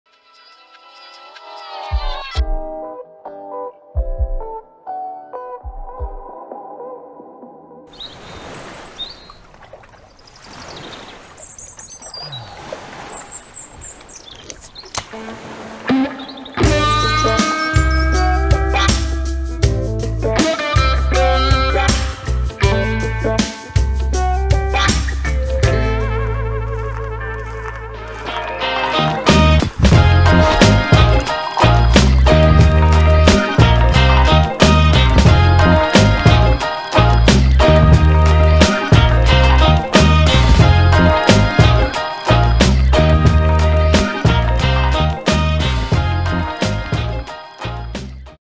Mines Bigger... Rhyme x Reason TVC by Bunker Street Film Co.
TVC for Rhyme x Reason brewers mini keg release.